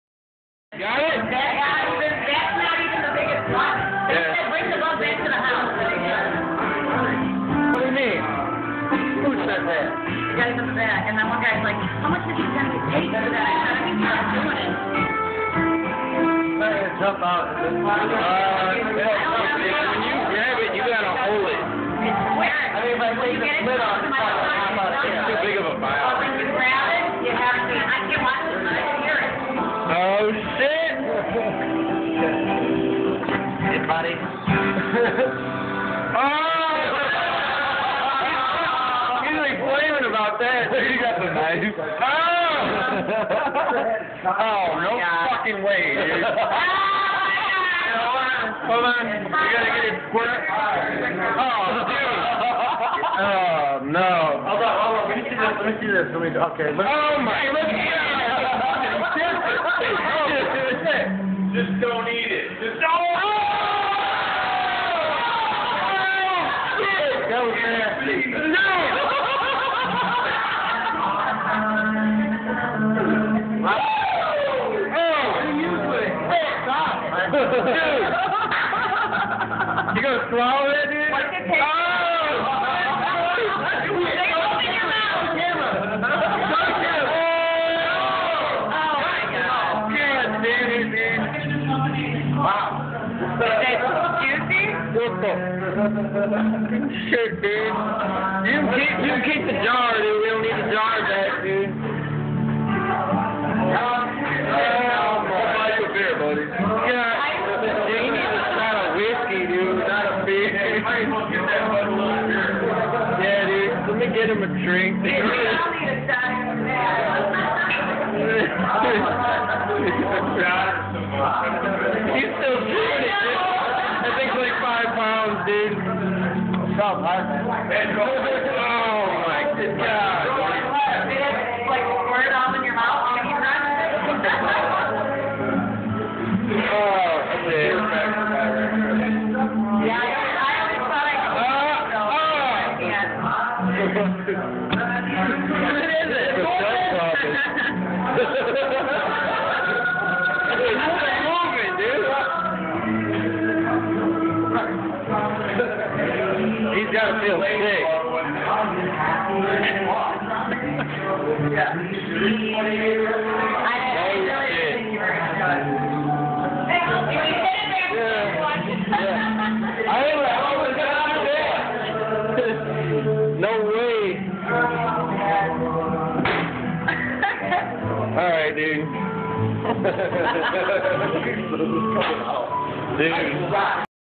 Guy At Bar Eats Huge LIVE Madagascar Cockroach!!!!!!
So I get a call from my sis that some dude wants to eat a Cockroach. We were nice and grabbed one of the medium ones. but the medium ones are 3 1/2 inches long! A little Ranch dressing and he's good to go! Check out this video i took of it all goin down!